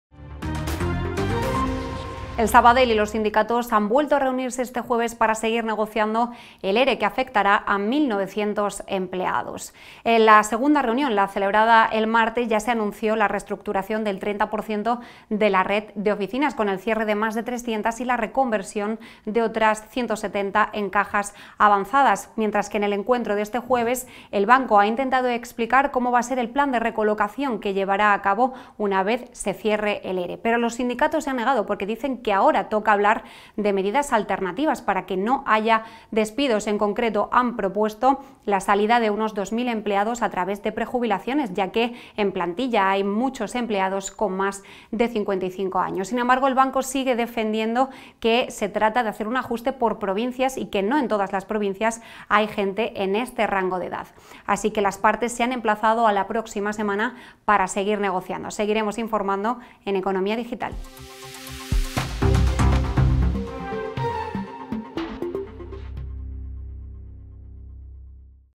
Los sindicatos del Sabadell proponen 2.000 prejubilaciones en lugar de hacer un ERE
Banc Sabadell se ha reunido este jueves con los sindicatos en su tercer encuentro desde que anunció un Expediente de Regulación de Empleo (ERE).

La reunión ha comenzado con la intervención de los representantes sindicales, quienes han propuesto medidas alternativas como prejubilaciones, bajas incentivadas y excedencias voluntarias.

Todos los sindicatos han defendido que las salidas deben centrarse exclusivamente en los mayores de 55 años, porque hay más de 2.000 personas en esta franja de edad, una cifra similar al excedente de personal calculado por el banco.

Te informa Cristina Hidalgo